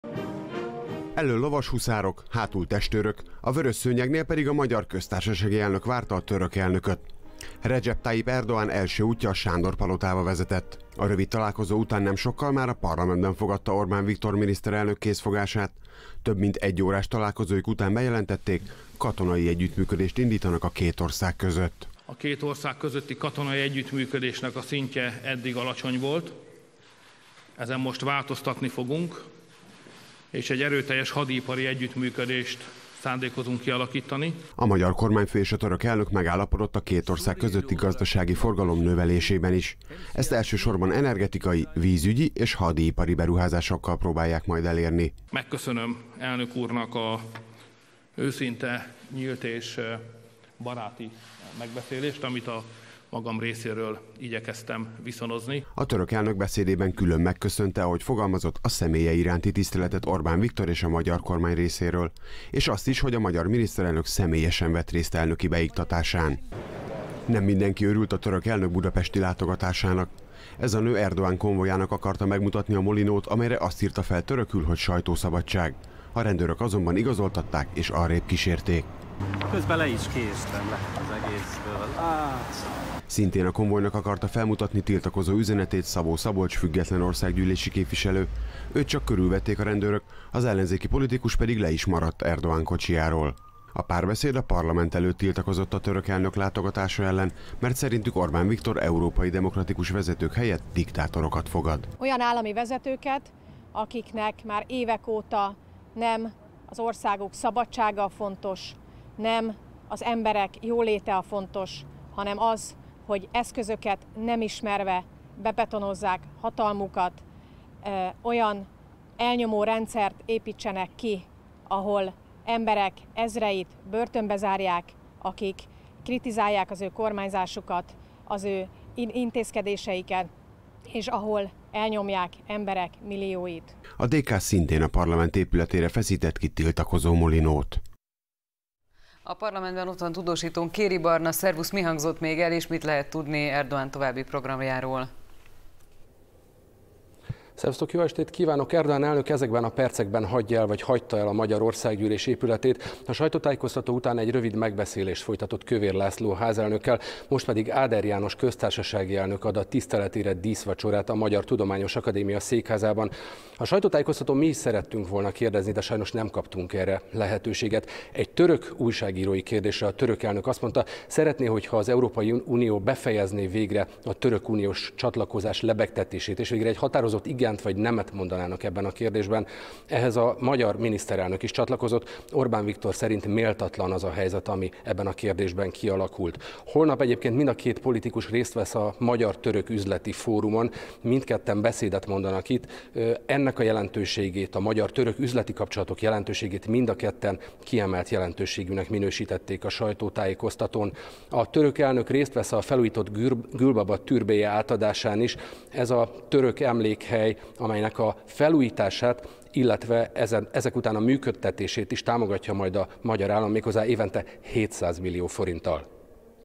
Erdogan első napja: tárgyalás és tiltakozás 18-10-08
Katonai együttműködésről tárgyalt Orbán Viktor Recep Tayyip Erdogan török elnökkel Budapesten. Megállapodtak arról is, hogy növelik a két ország közötti gazdasági forgalmat, például energetikai, vízügyi és hadiipari beruházásokkal. A török elnök kétnapos látogatásának első napján Orbán Viktor mellett találkozott Áder János államfővel is. Erdogan érkezése ellen több ellenzéki párt is tiltakozott.